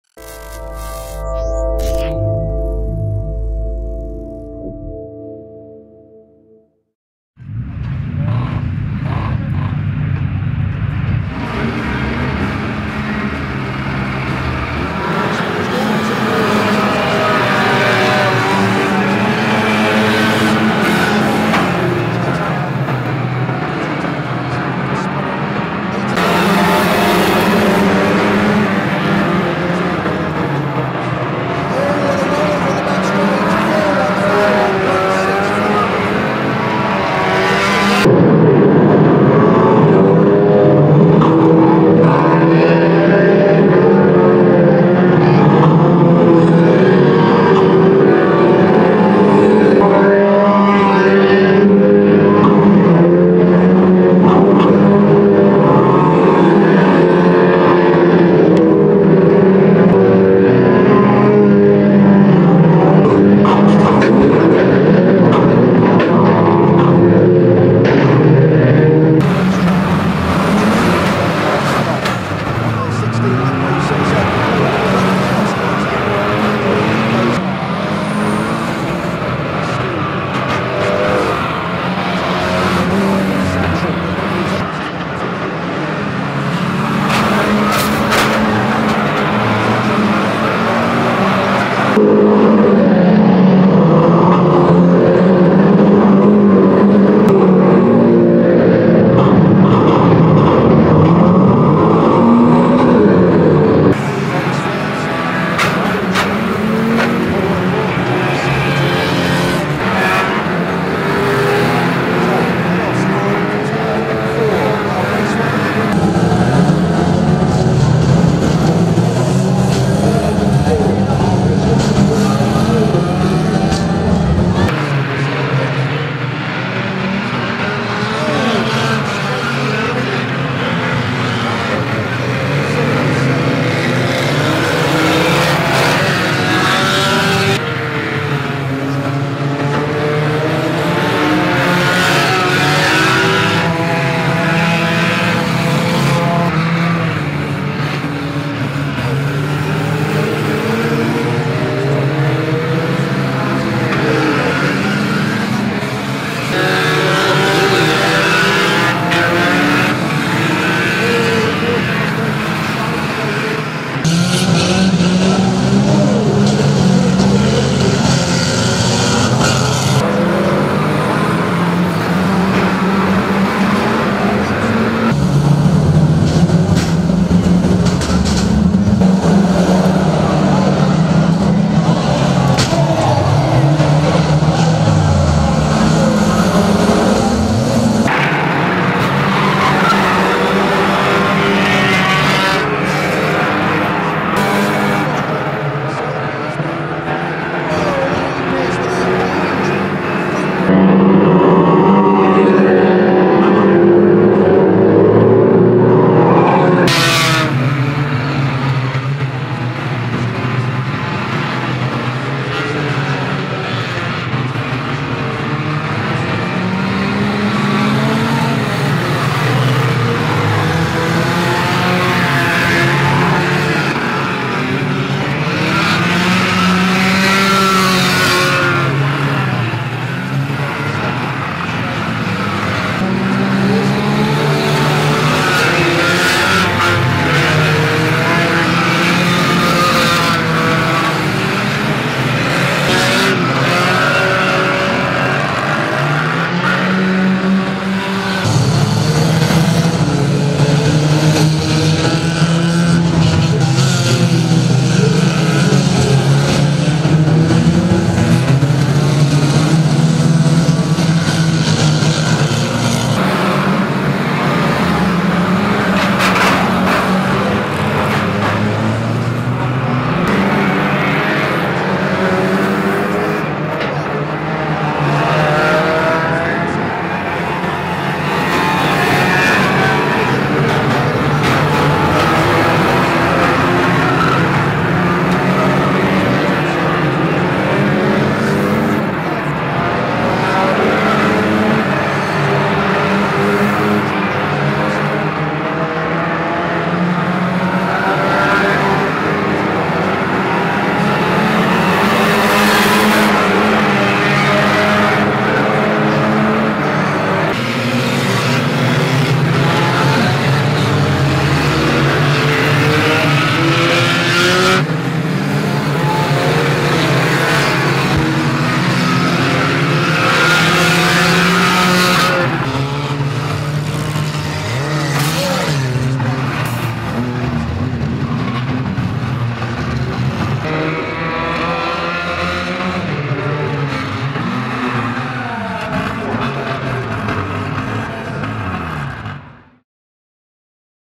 Mildenhall Micro Bangers and Stock Cars Jubilee Event 2022 Impact Videos
A selection of highlights from our multi camera mix of the Mildenhall Jubilee event on 2nd June 2022. Featuring Micro Bangers and Stock Car Racing. The full DVD production features every race with the live race commentary, race results, pre meeting pit shots, wreck shots and action replays from the various camera angles.